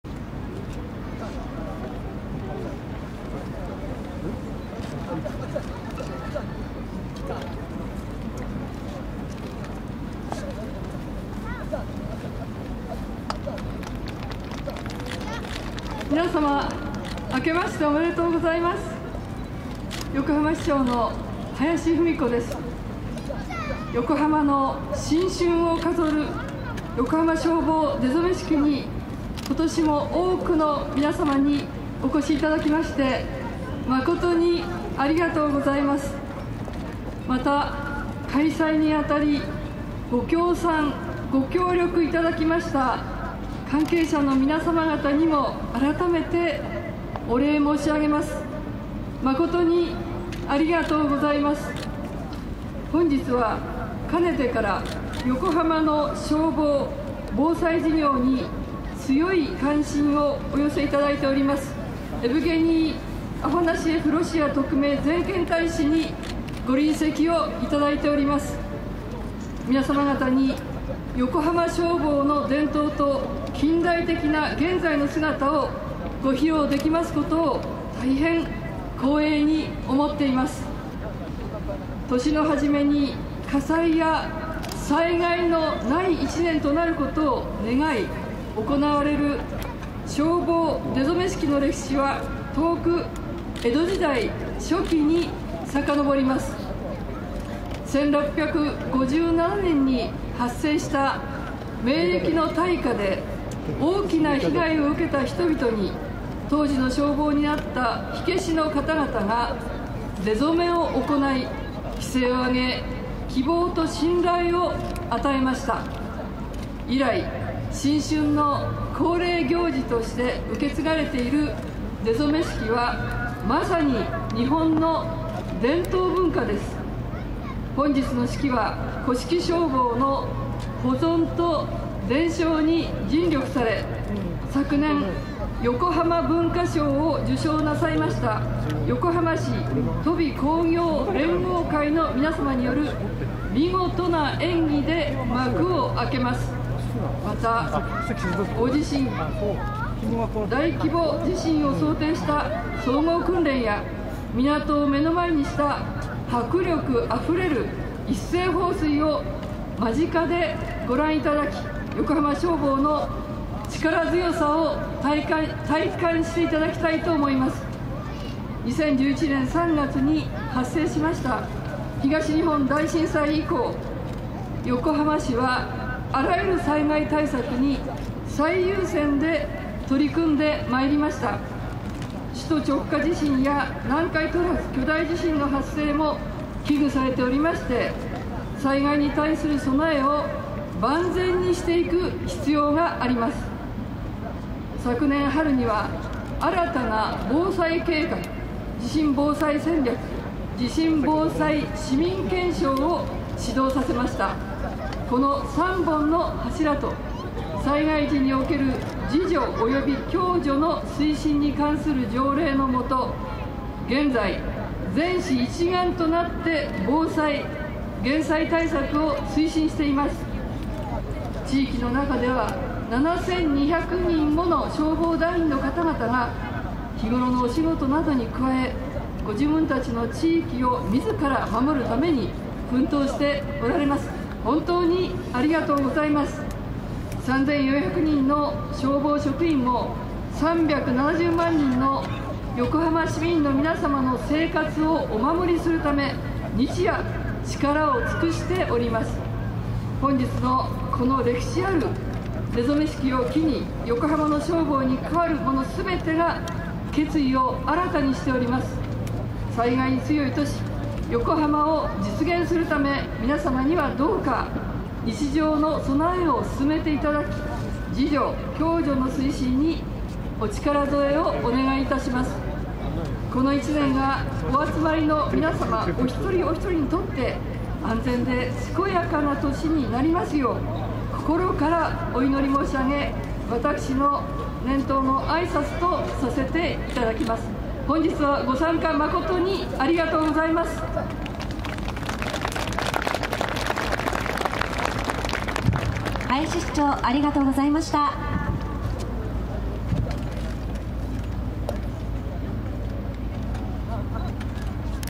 横浜消防出初式2014 「オープニングセレモニー」：林文子横浜市長の挨拶
2014年1月26日横浜赤レンガ倉庫イベント広場・１号館、赤レンガパークで開催された横浜消防出初式2014第１部のオープニングセレモニーでの林文子横浜市長による挨拶。